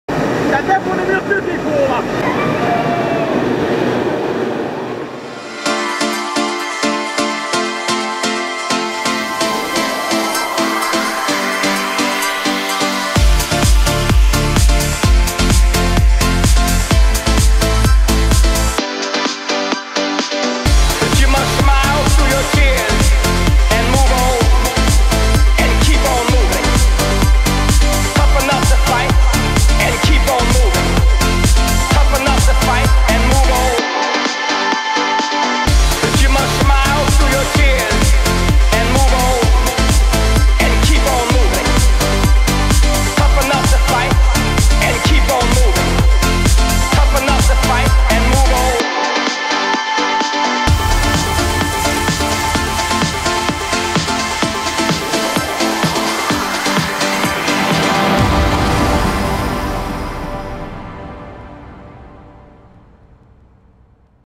Heikki L & Orkidea b2b @ Waterland 2014
Short clip from the Heikki L & Orkidea back2back set at Waterland 2014 in Serena. Friday was a blast and tonight we'll host Orkidea & Friends arena in the rave cave.